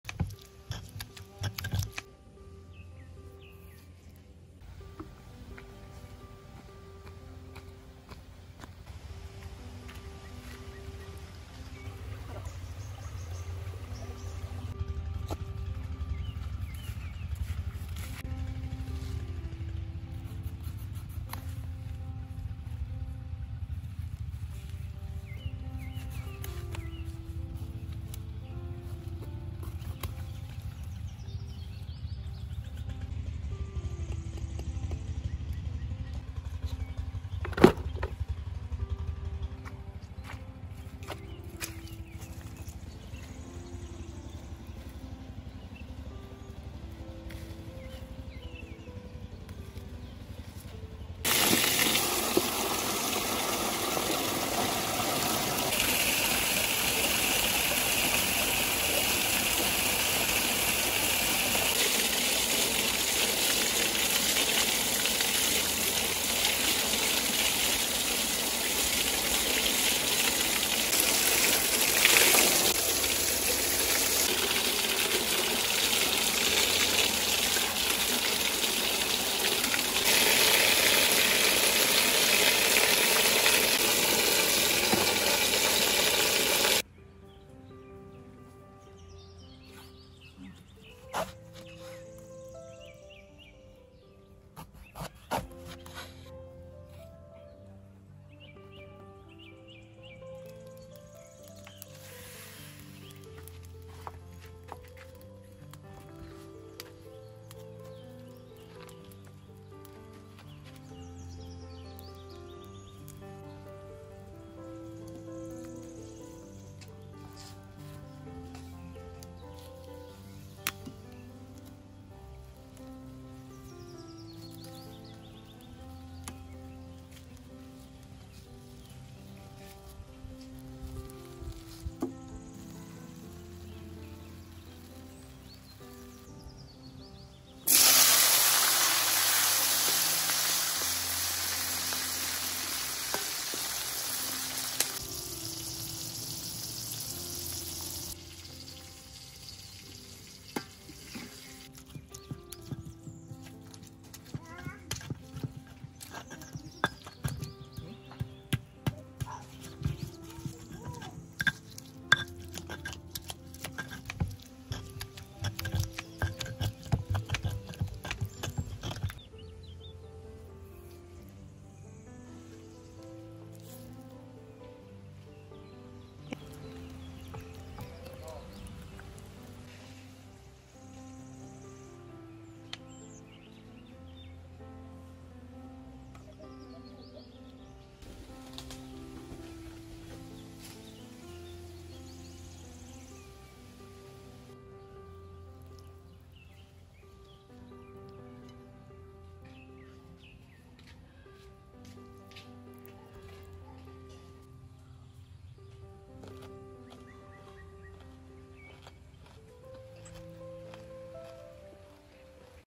Sambal terong bakar
Terong bakar sambal
Masak dikebun
Panen terong